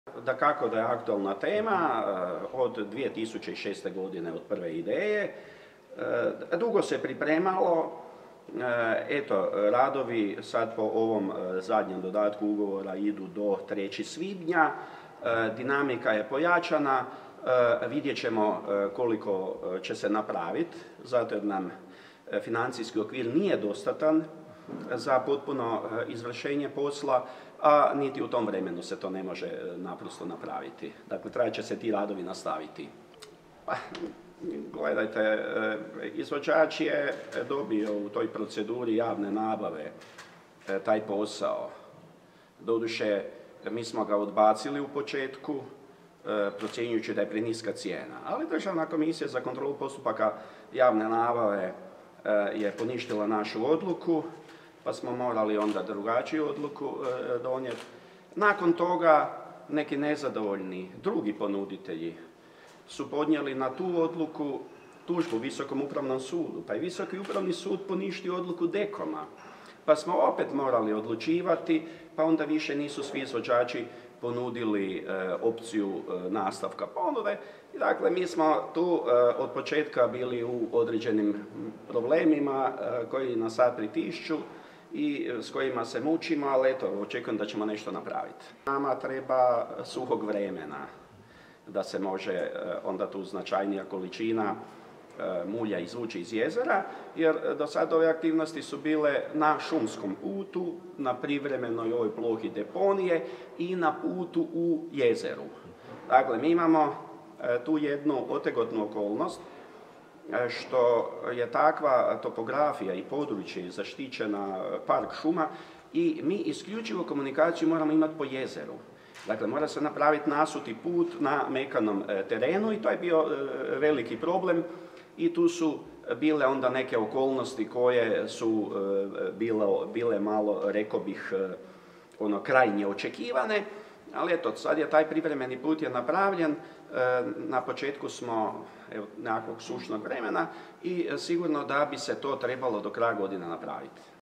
Danijel Bunić o kašnjenu radova na izmuljivanju jezera Trakošćan
U Areni Varaždin održana je konferencija "Projekt unaprijeđenja negrađevinskih mjera upravljanja rizicima od poplava u Republici Hrvatskoj".